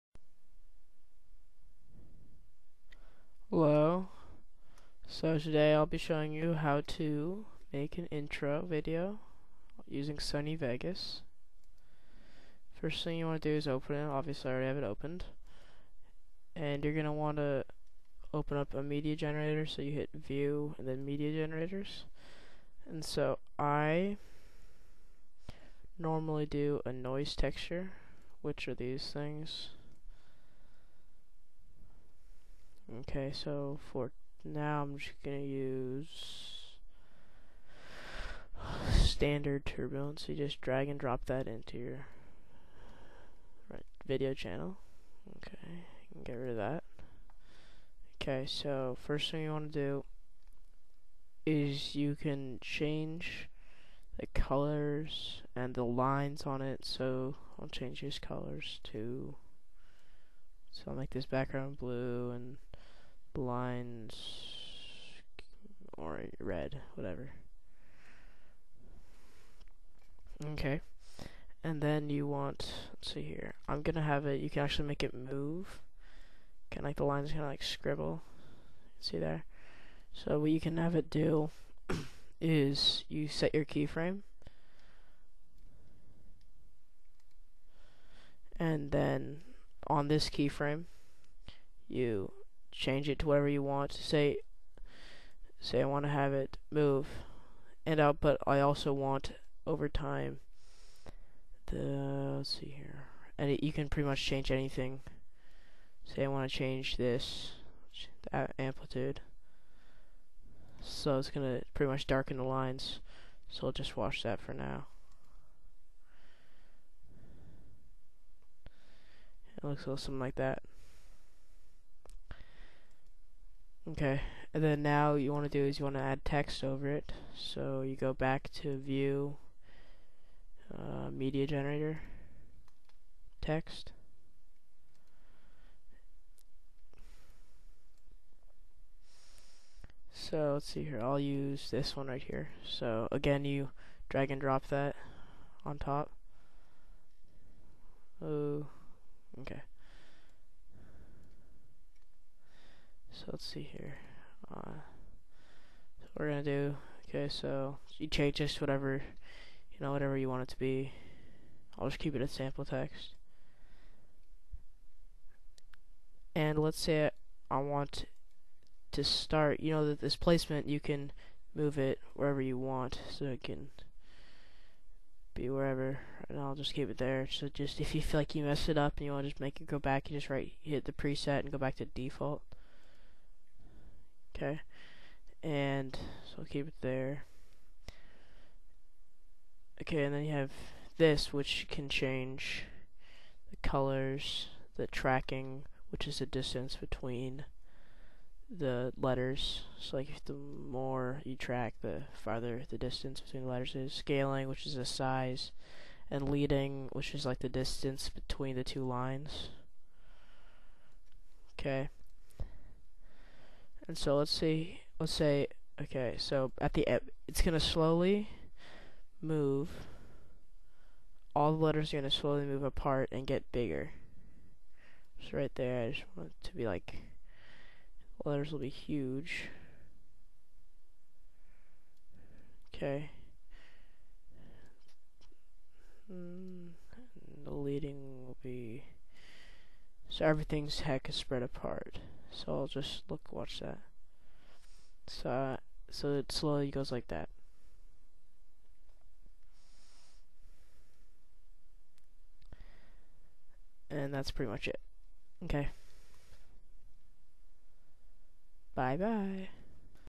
intro video tutorial :: sony vegas
***Request any sony vegas tutorial***

a brief tutorial on how to make intro videos using sony vegas